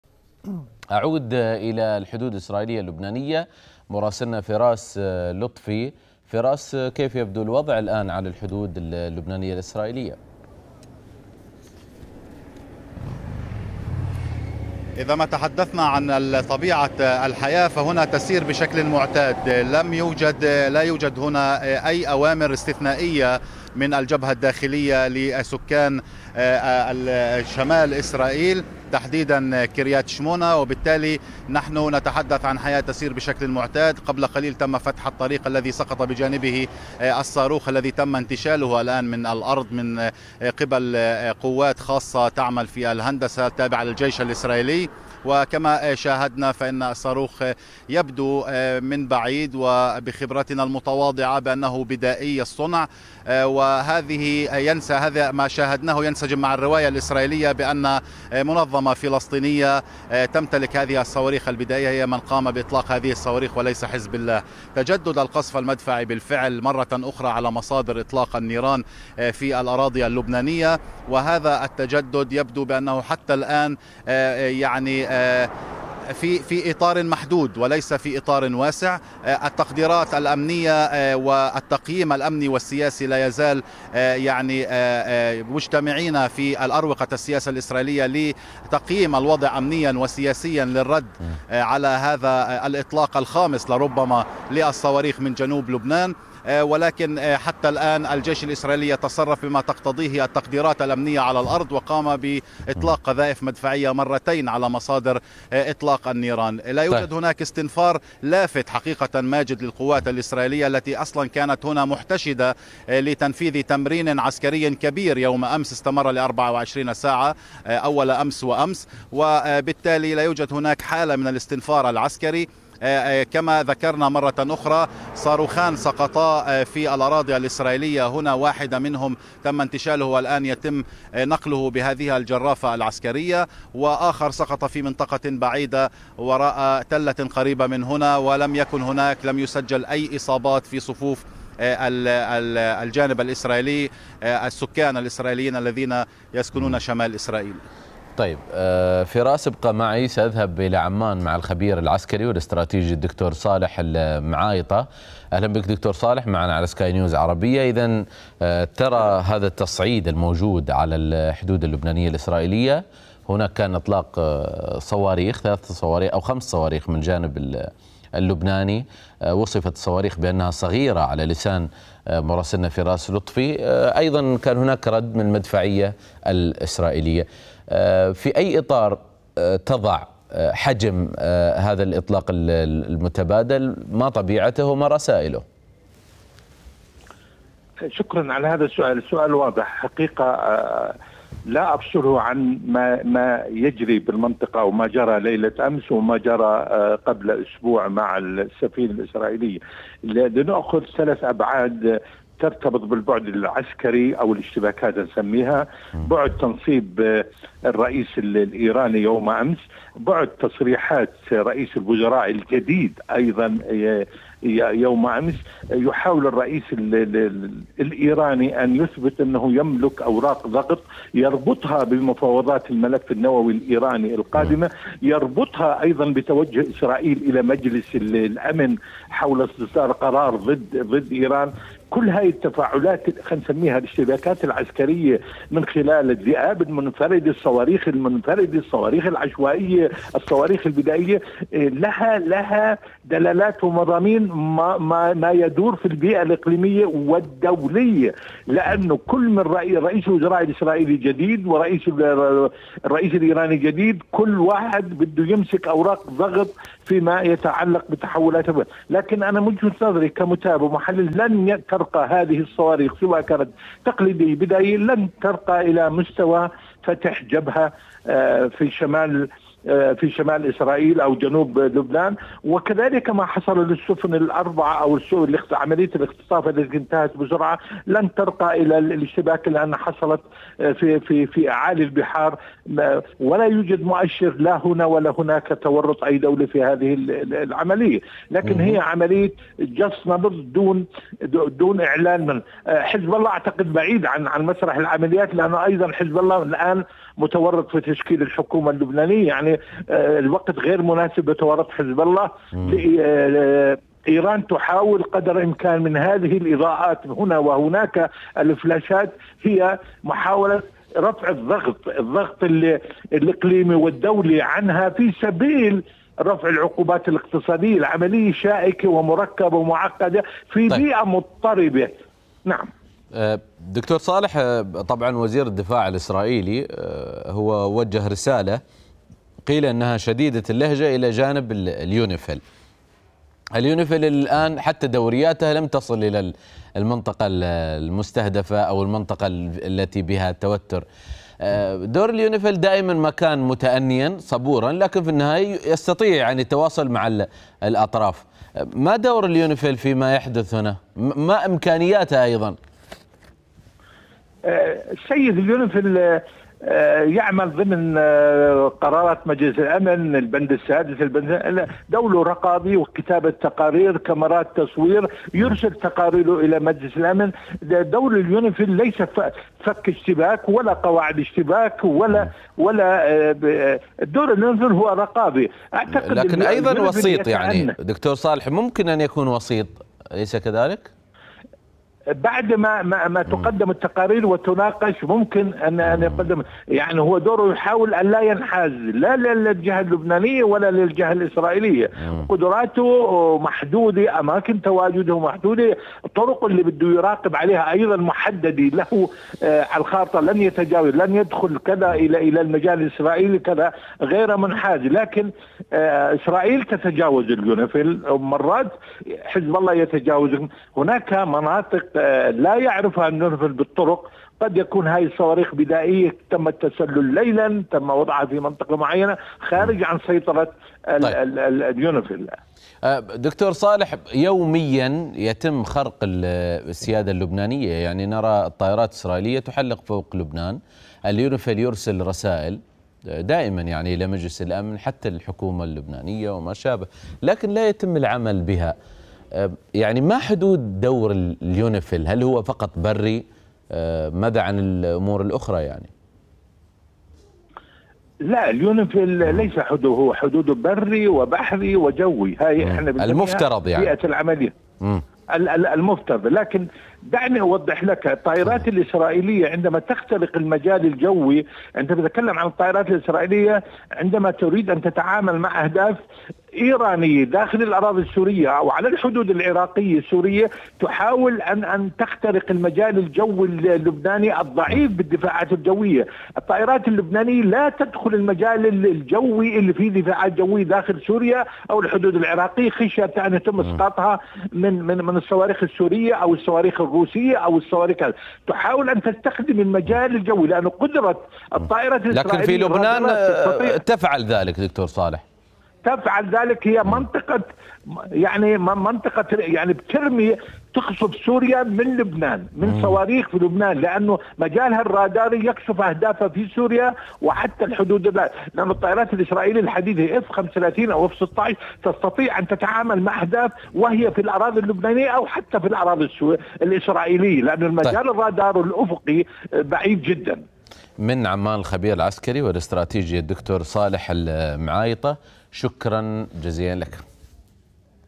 عاجل.. المدفعية الإسرائيلية تطلق النيران مجددا باتجاه الأراضي اللبنانية
شن جيش الاحتلال الإسرائيلي قصفا مدفعيا على داخل الأراضي اللبنانية، زاعما أنها جاءت ردا على إطلاق 3 صواريخ من لبنان سقط أحدها في الأراضي اللبنانية في حين سقط آخران في إسرائيل، مشيرا إلى أن صافرات الإنذار كانت تدوي في بلدات كريات شمونة، كفار جلعادي وتل حي.